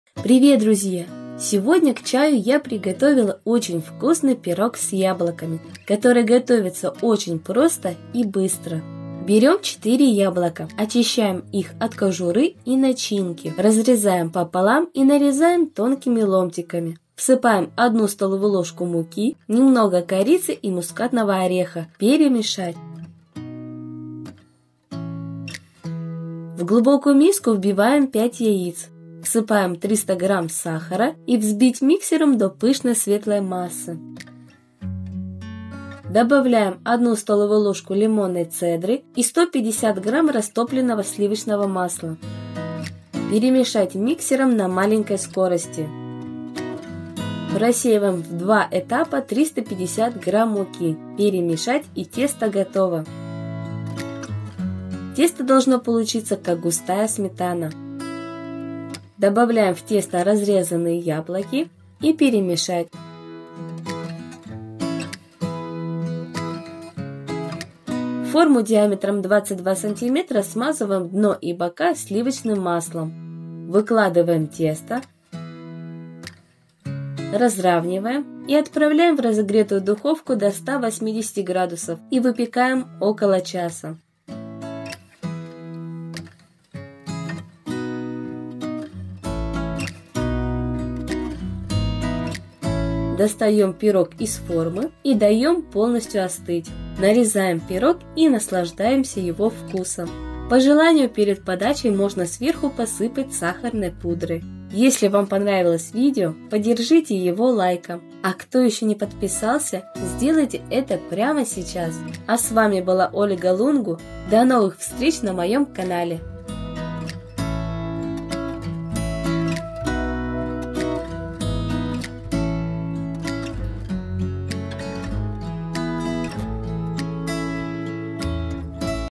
Пирог "ШАРЛАТАНКА" с яблоками без разрыхлителя и дрожжей
Сегодня я приготовила БЕСПОДОБНУЮ ШАРЛОТКУ с яблоками, а  так же ёё называют "Шарлотанка" с яблоками Такая шарлотка готовится без разрыхлителя и дрожжей. В таком пироге я люблю когда много яблок.

Ингредиенты:

Масло сливочное растопленное — 150 г.
Сахар — 300 гр.
Корица — по вкусу
Орех мускатный — по вкусу
Лимонная цедра — 1 ст.л.